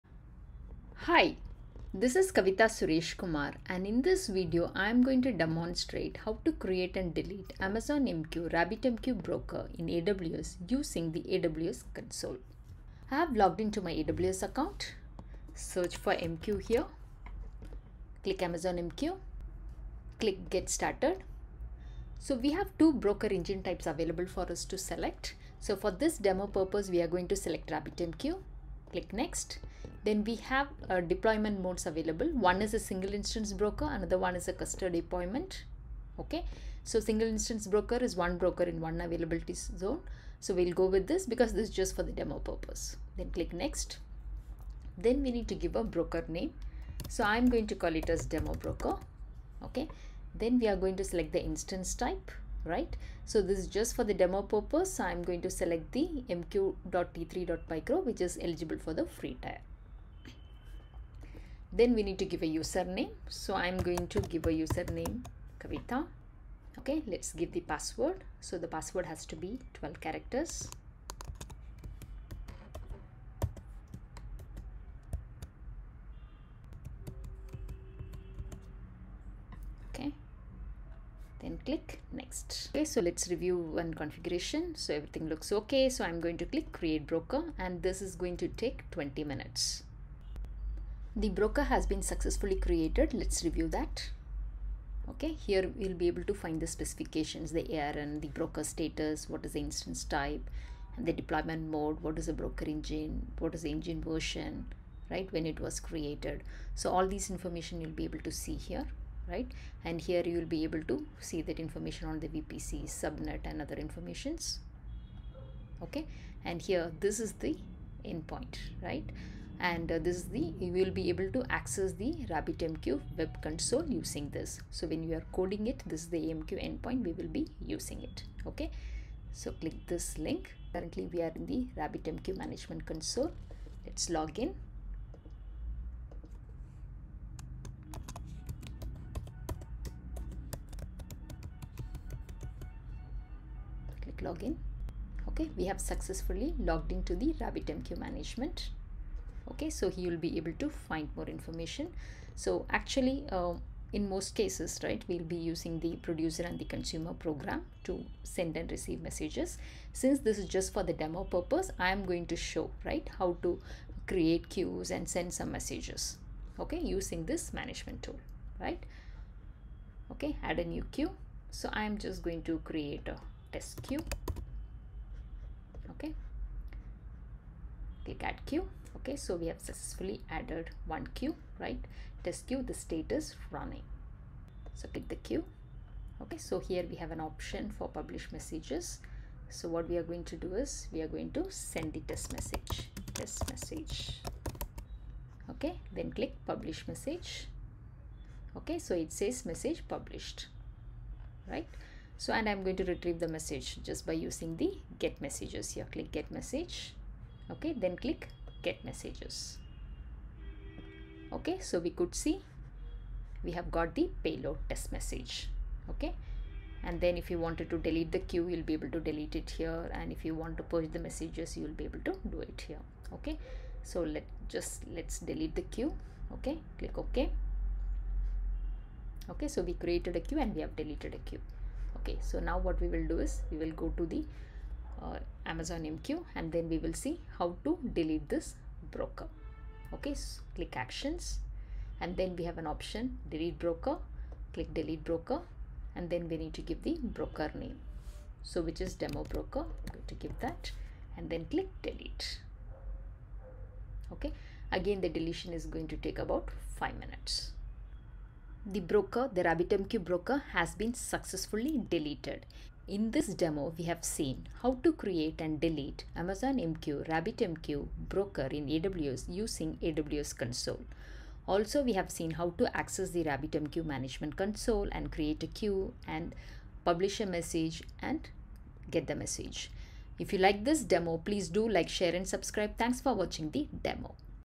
Creating and deleting Amazon MQ - RabbitMQ broker in AWS Using Console
This video will teach you how to create and delete Amazon MQ - RabbitMQ broker in AWS Using Console. Also, how to access the RabbitMQ Management Console, create a queue, publish, and get messages.